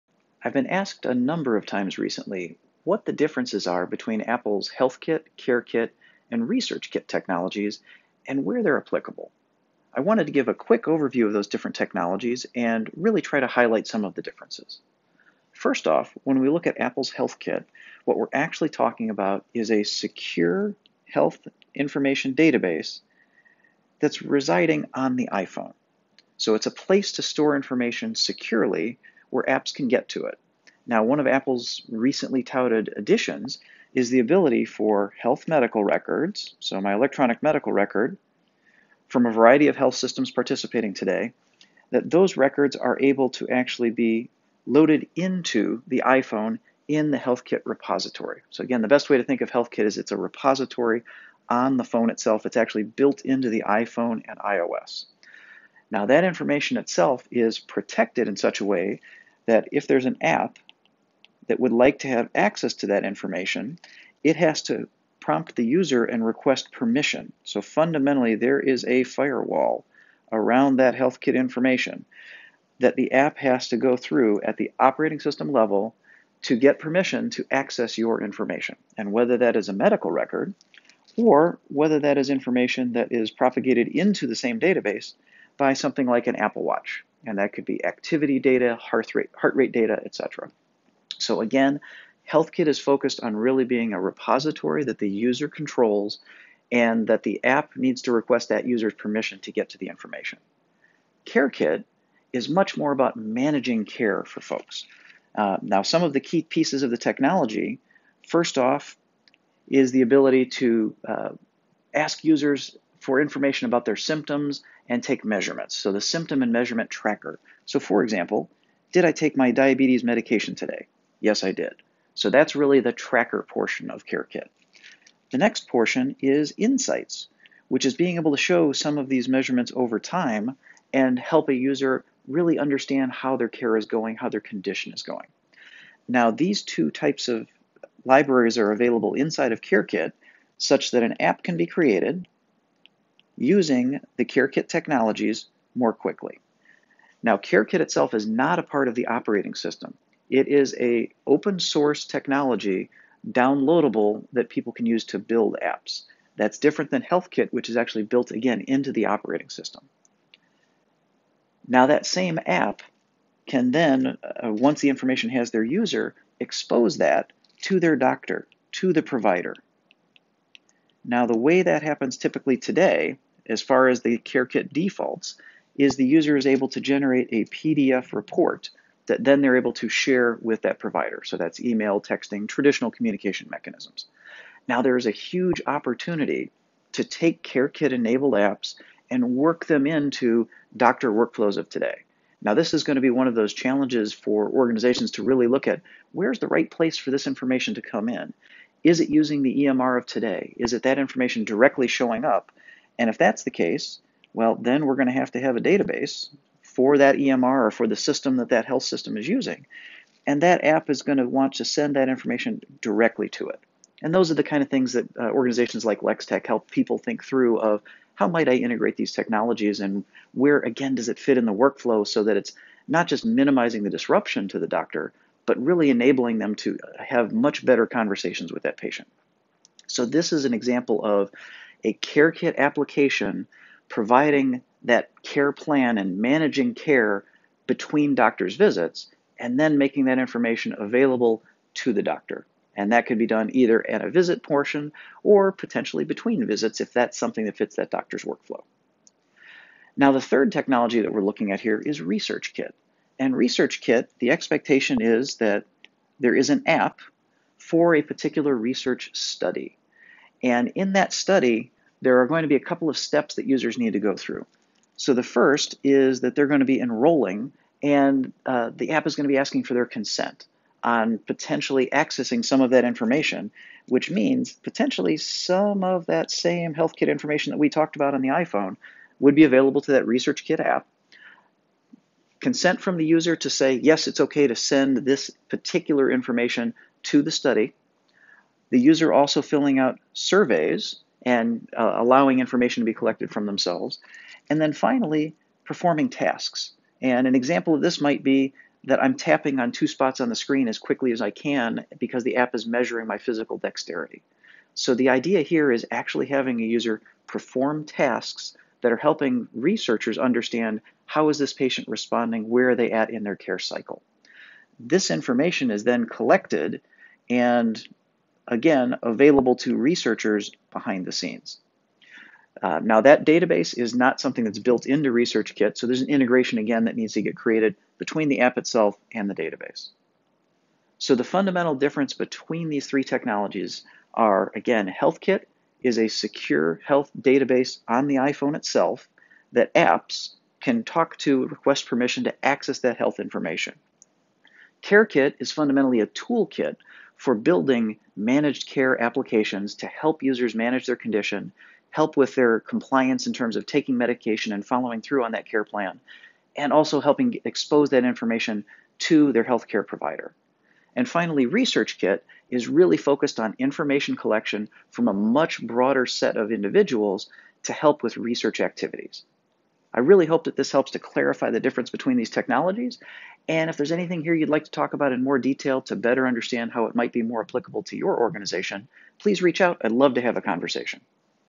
How Apple's HealthKit, CareKit, & ResearchKit Are Changing The Way We Manage Our Health
I've been asked many times recently about the difference between Apple's HealthKit, CareKit and ResearchKit and where they should be used. I recorded this quick overview (on my iPad of course) to help clarify how these technologies are changing the way we all manage our health. Please post comments if you have any questions I didn't cover.